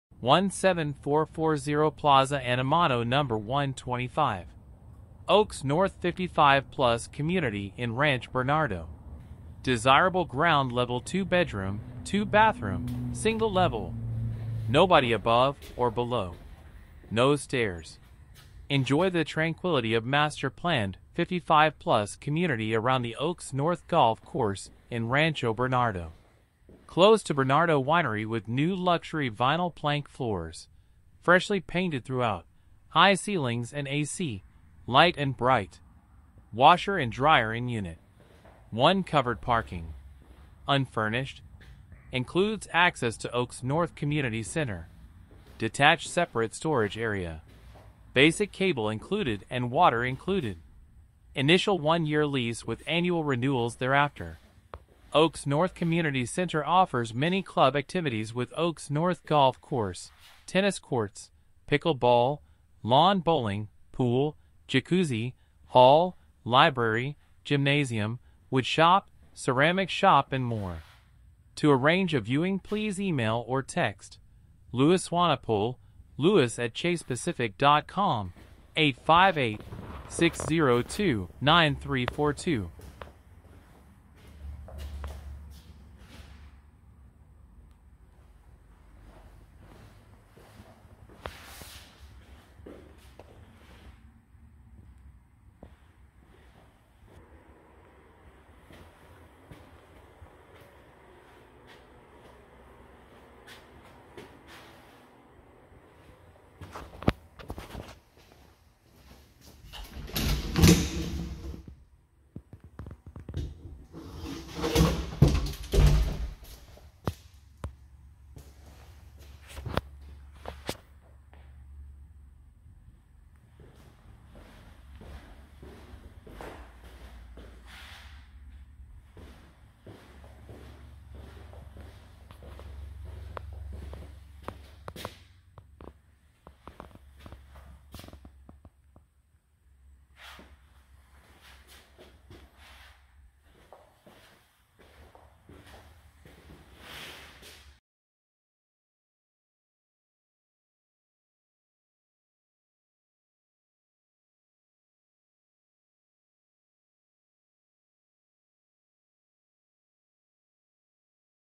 17440 Plaza Animado #125 video 2
17440 Plaza Animado #125
Oaks North 55+ Community in Ranch Bernardo. Desirable ground level 2 bedroom, 2 bathroom, single level. Nobody above or below. No Stairs. Enjoy the tranquility of master planned 55+ community around the Oaks North golf course in Rancho Bernardo. Close to Bernardo Winery with new luxury vinal plank floors, freshly painted throughout, high ceilings and AC. Light and bright. Washer and dryer in unit. One covered parking. Unfurnished. Includes access to Oaks North Community Center. Detached separate storage area. Basic cable included and water included. Initial one year lease with annual renewals thereafter.
Oaks North Community center offers many club activities with Oaks North golf course, tennis courts, pickle ball, lawn bowling, pool, jacuzzi, hall, library, gymnasium, wood shop, ceramic shop and more.